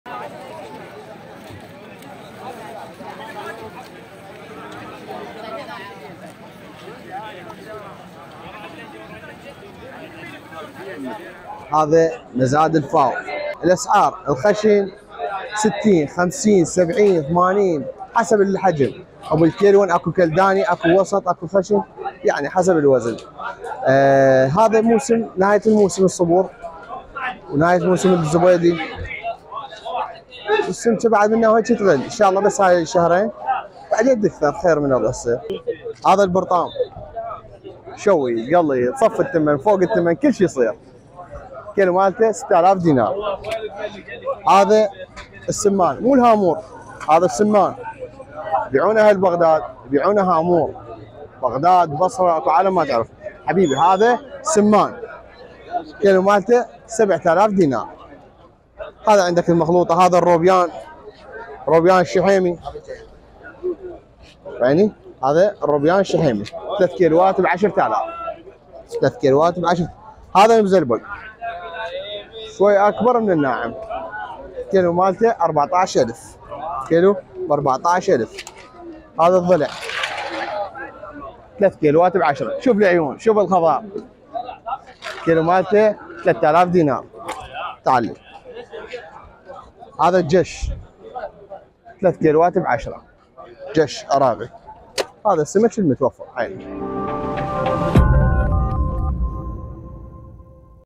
لا صبور بعد اليوم في مزاد الفاو وقوارب الصيد تعود من البحر بأسماك نادرة فيديو
مع انتهاء موسمه ورغم ارتفاع سعره، ازداد الإقبال على شراء الصبور، اليوم الأحد، في سوق النقعة جنوبي البصرة، فيما استعرض الصيادون أسماك نادرة من البحر، أبرزها الزبيطي والشعري.

أسعار السمك في مزاد الفاو الصباحي ليوم الأحد، وفقاً للصياد مصطفى نعمان:
زبيطي 8 آلاف د. ع – ارتفع 2000 د. ع عن سعر يوم أمس السبت.
شعري 4 آلاف د. ع – انخفض 1000 د. ع
سمك السگن 3 كيلوات 10 آلاف د. ع
برطام 6 آلاف د. ع – انخفض 1000 د. ع
سمك السمان 7 آلاف د. ع – ارتفع 2000 د. ع
روبيان شحيمي 3 كيلوات 10 آلاف د. ع – انخفض 1000 د. ع
روبيان مزلبگ 14 الف د. ع – انخفض 1000 د. ع
ضلع 3 كيلوات 10 آلاف د. ع – انخفض 1000 د. ع
چش 3 كيلوات 10 آلاف د. ع – انخفض 1000 د. ع